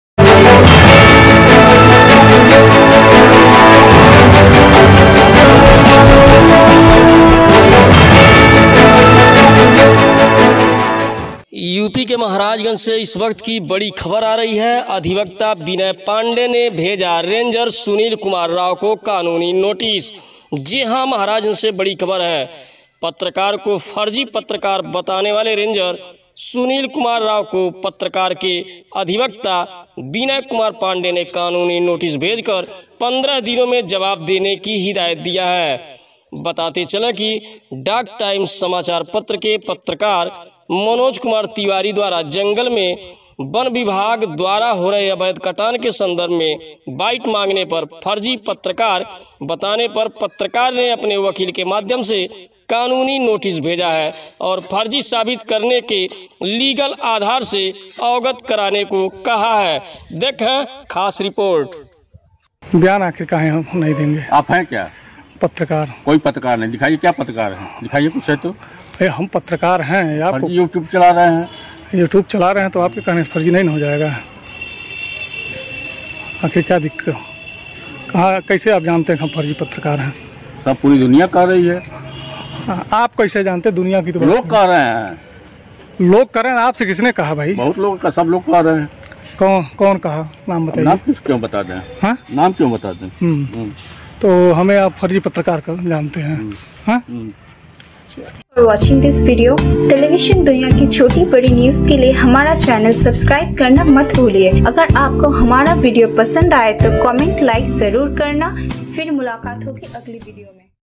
Nichlaul:बेलगाम रेंजर ने कहा फर्जी पत्रकार, अधिवक्ता विनय कुमार पांडेय ने भेजा कानूनी नोटिस Breaking
india hindinews # Nichlaul:बेलगाम रेंजर ने कहा फर्जी पत्रकार, अधिवक्ता विनय कुमार पांडेय ने भेजा कानूनी नोटिस